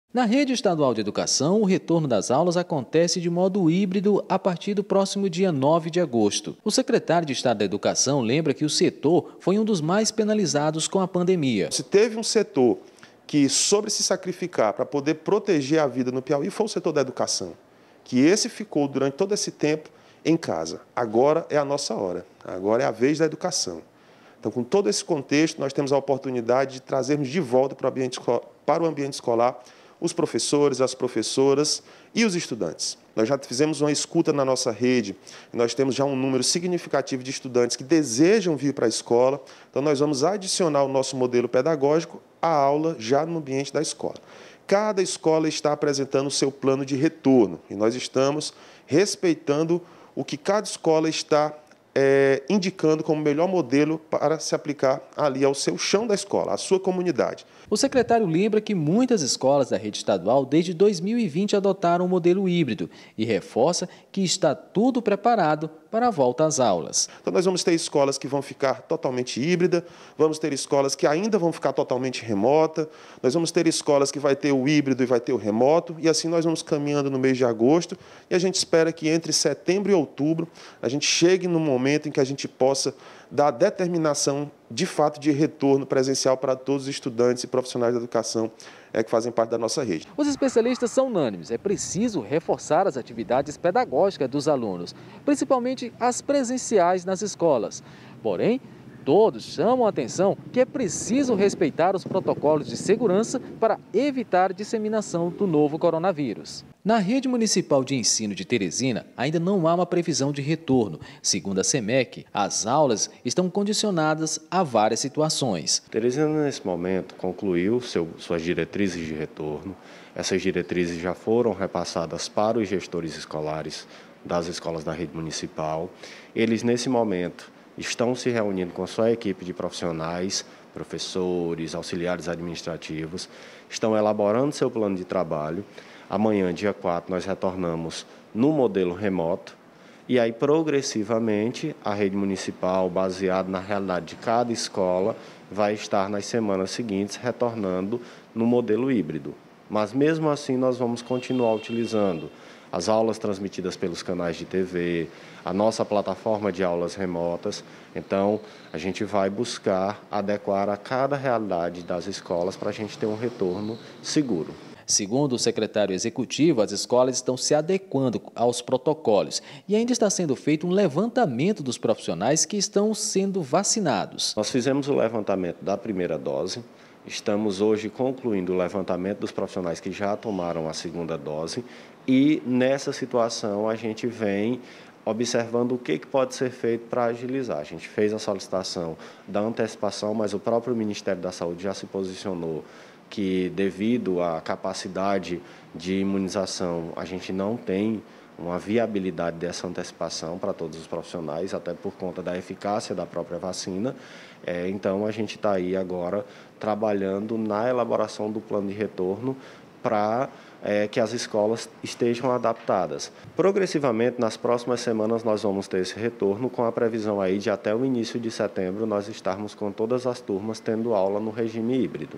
Retorno das aulas presenciais é controverso no Piauí
O retorno das aulas presenciais nas escolas públicas voltou a ser tema de discussão no estado do Piauí. Com o avanço da vacinação contra a Covid-19 e redução no número de casos, alguns especialistas já recomendam a retomada das aulas, mantendo os cuidados necessários para a saúde. Veja mais detalhes na reportagem de James Almeida e Roniel Matos, da TV Assembleia.